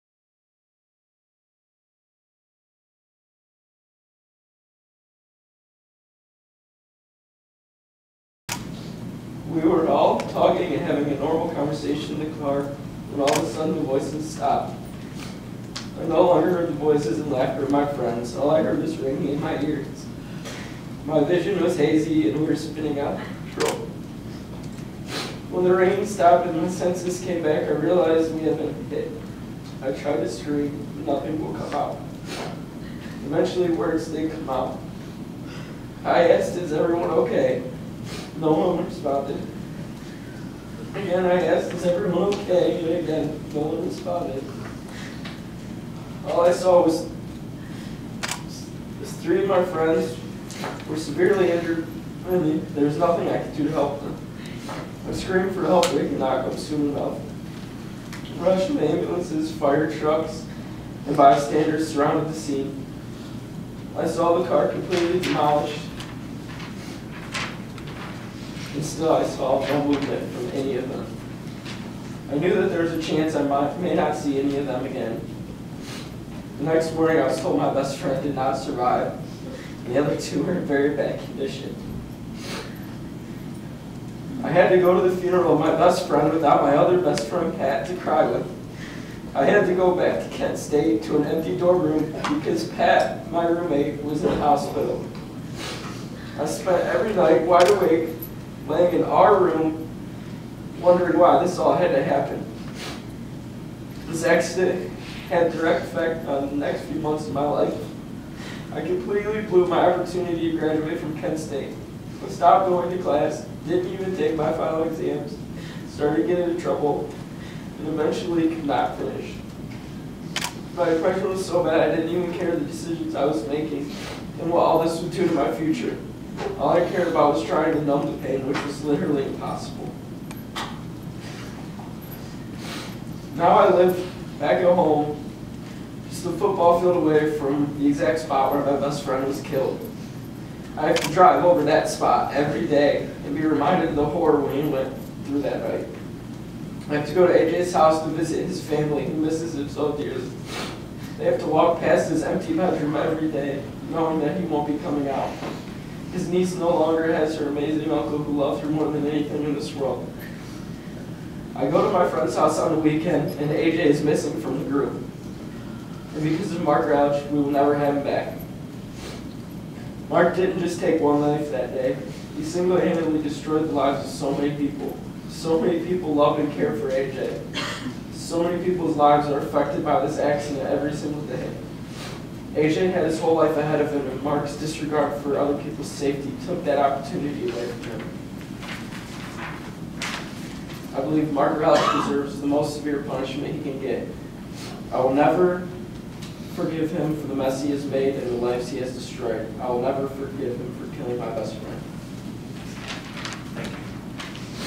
Cody Toboz talks about the accident that killed his friend and severely injured him
A November 2010 accident killed Elyria man AJ Vincent and critically injured three other people. The drunk driver, Mark Ralich was sentenced to 7 years.